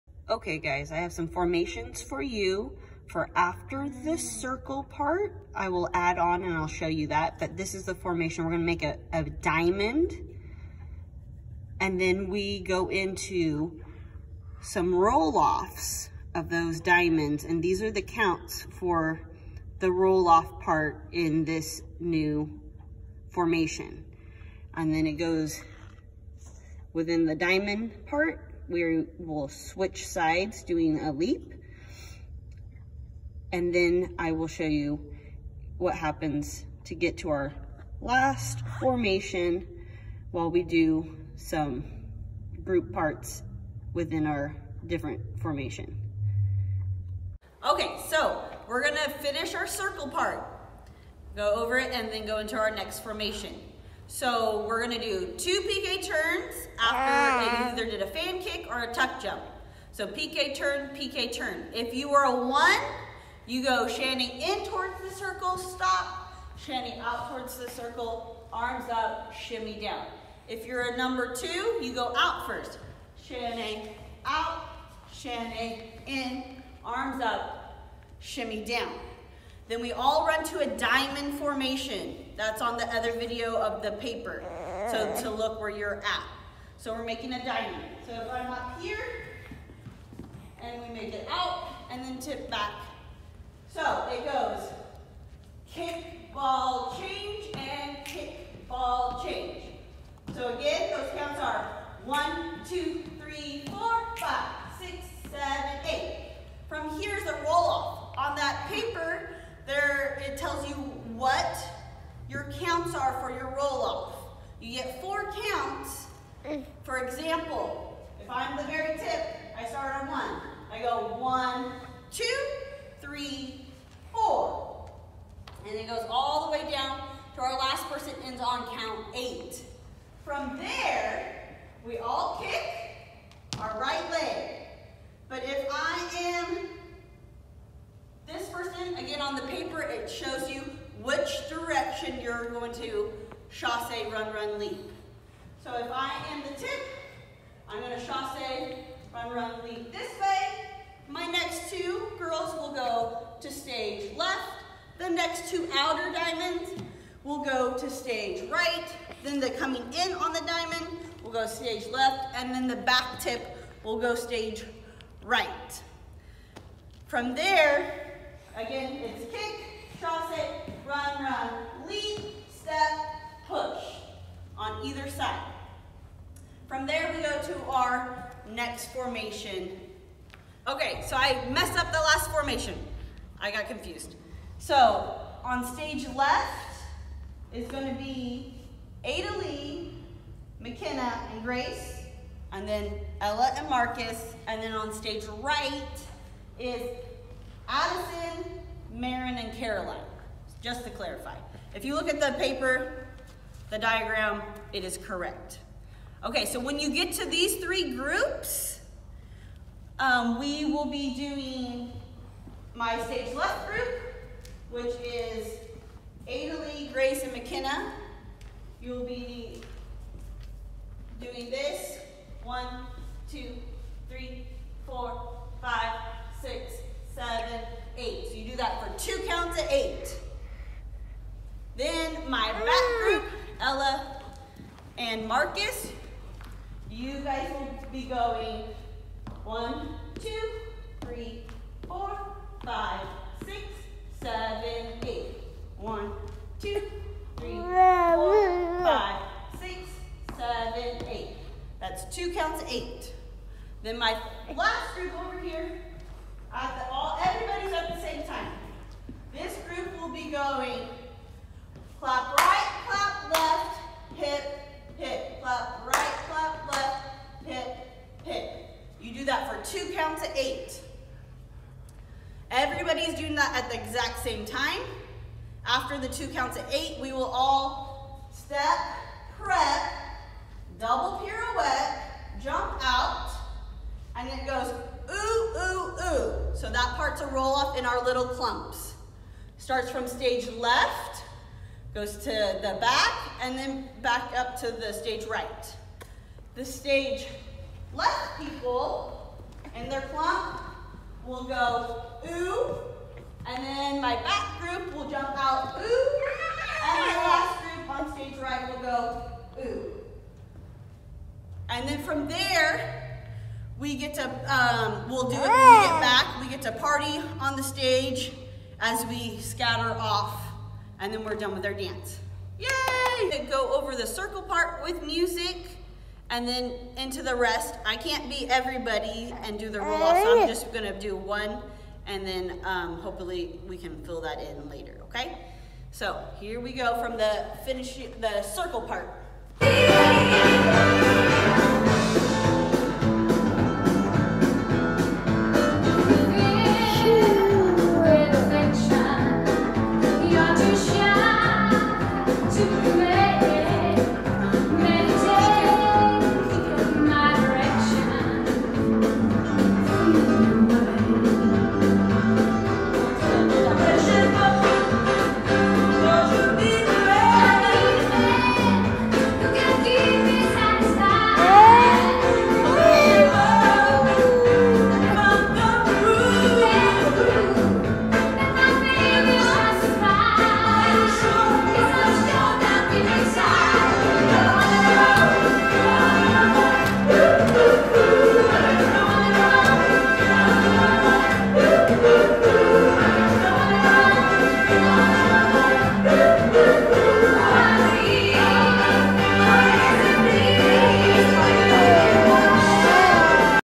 Miss Tara’s Jazz 4 Class
Miss Tara shares choreography to work on for her Jazz 4 class that meets on Wednesday nights from 5:45-6:45pm in Studio A.